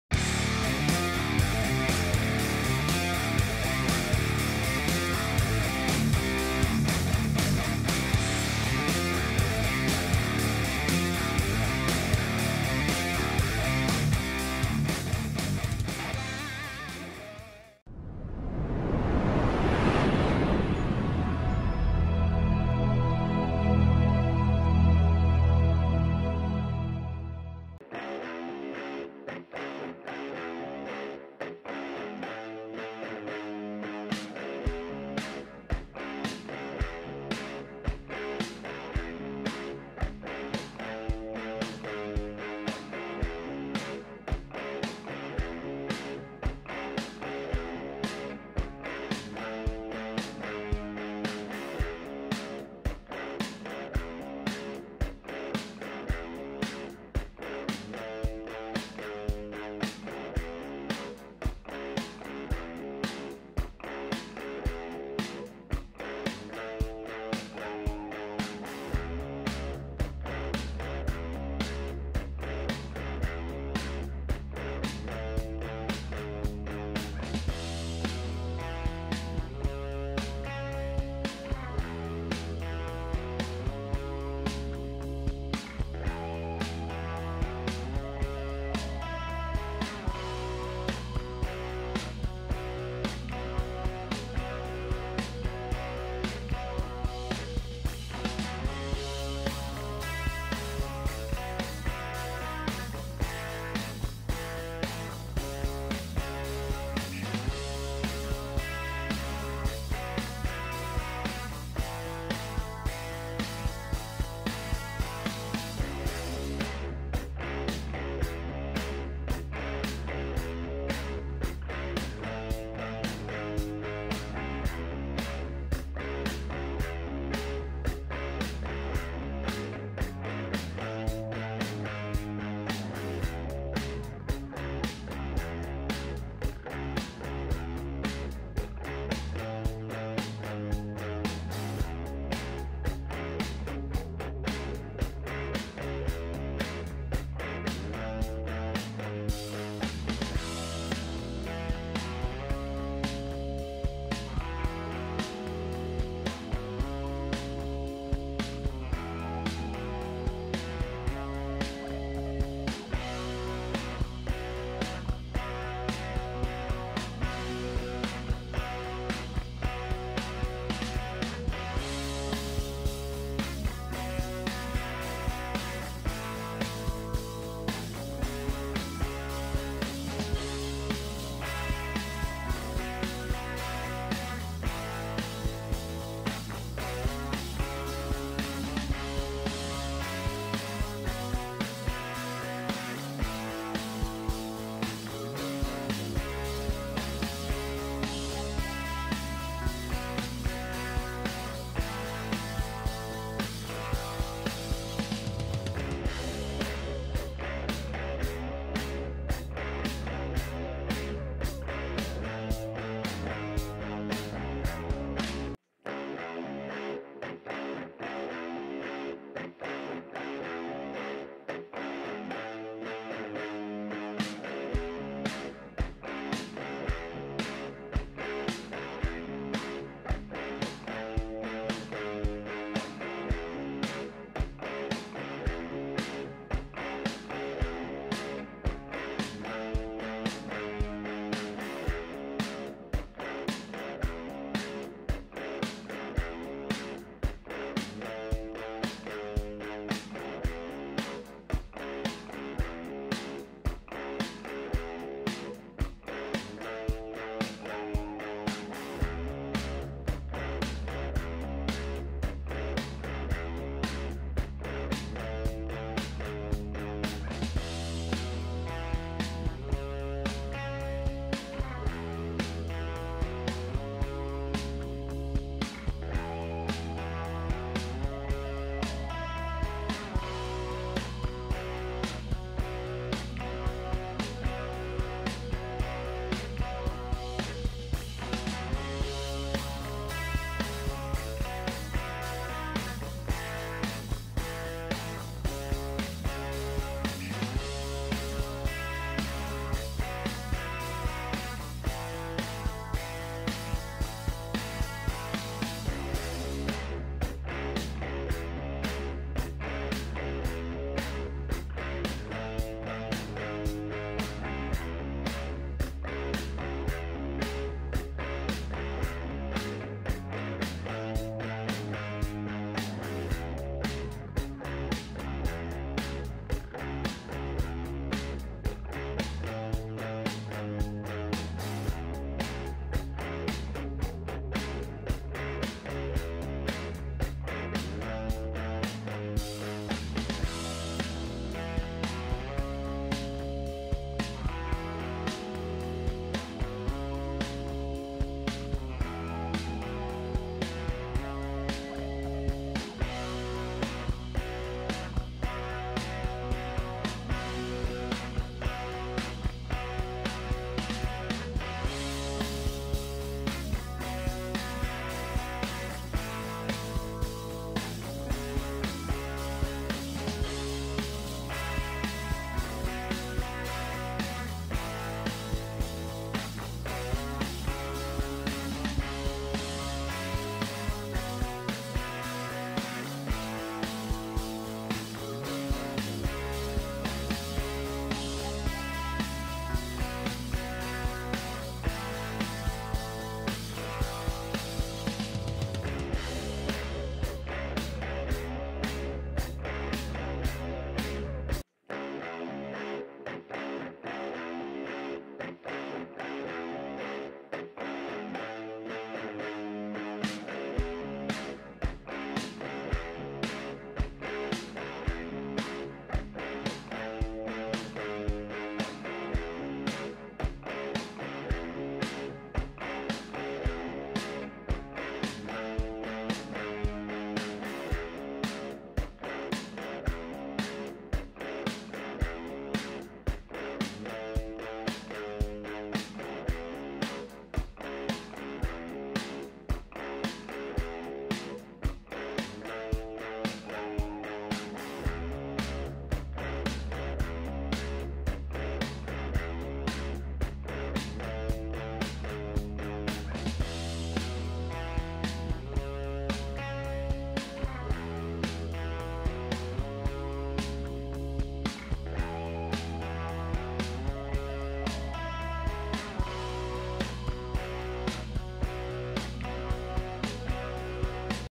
Como Limpiar Monedas Oxidadas
El Fin Del Video Es Poder Limpiar Rapidamente Las Monedas Que La Mayoria De Los Colegas Detectoristas Encuentran Con Sus Detector De Metales . Las Monedas Que Se Limpian En El Video Son Todas De Curso Actual Sin Valor Numismática Alguna .